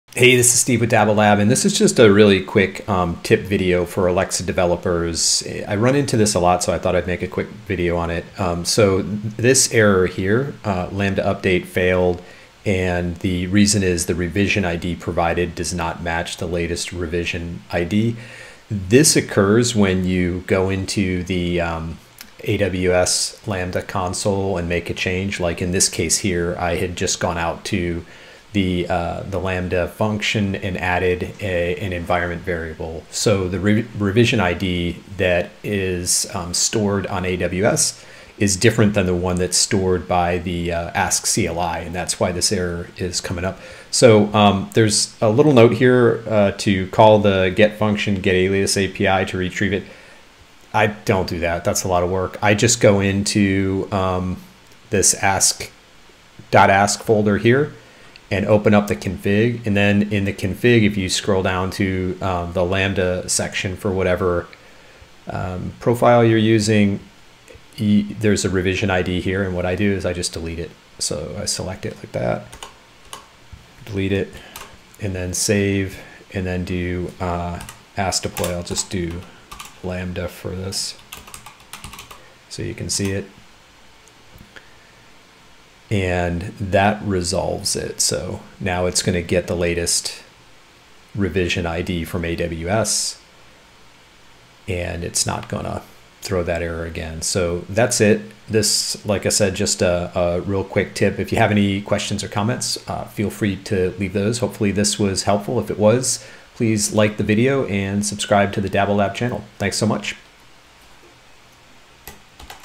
Resolving the ASK CLI Lambda Revision ID error - Dabble Lab #205
#205: This quick video will show you how to resolve the ASK CLI error: 'The Revision Id provided does not match the latest Id.'

≡≡ 100+ MORE TUTORIALS  ≡≡